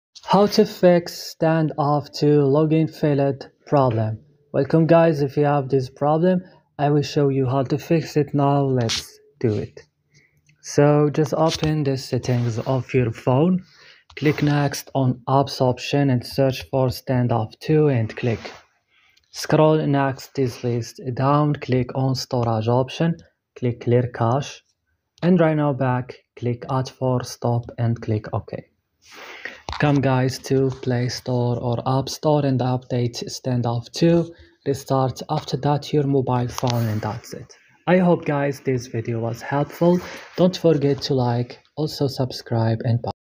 How To Fix Standoff 2 Game App Login Failed | Step By Step
inapp